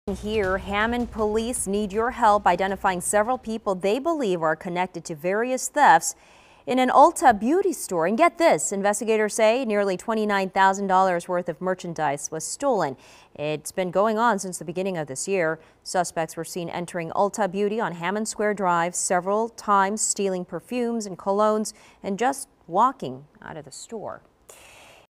Have you seen them? Suspects sought for Ulta Beauty thefts in Hammond
Hammond police are looking for several people wanted in connection with thefts reported at an Ulta store.